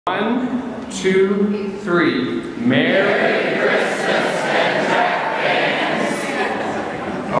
Merry Christmas from the Ventrac Employees
From all the Ventrac employees to you, Merry Christmas! This was filmed at our 2015 Christmas Banquet.